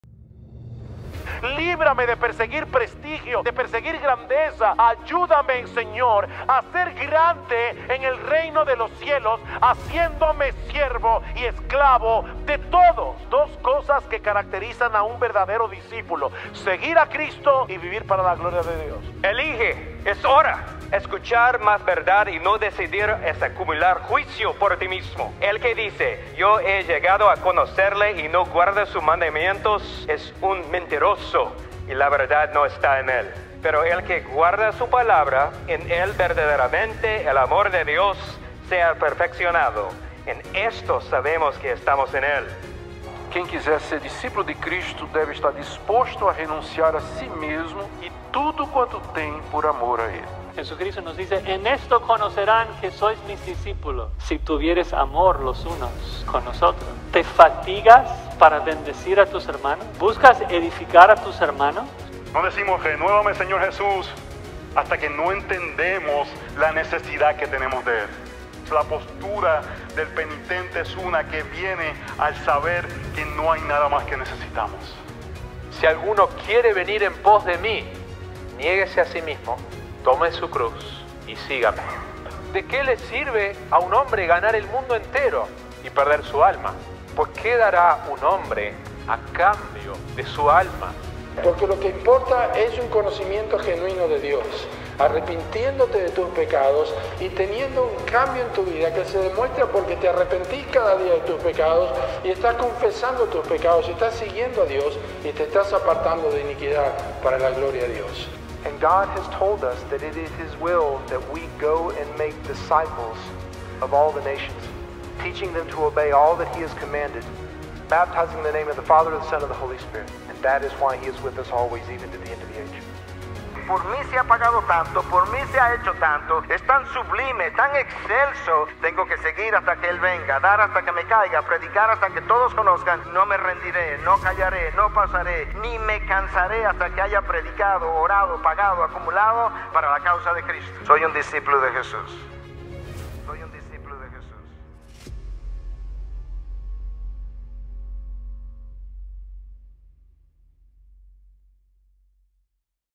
Discipulado Eficaz - Conferencia Ante Su Palabra 2021
Del 2 al 5 de Febrero podrás disfrutar de manera online la conferencia Discipulado Eficaz con invitados como Augustus Nicodemus, Miguel Núñez, Sugel Michelén,  Josías Grauman, Joselo Mercado y muchos más.
Abierta la inscripción.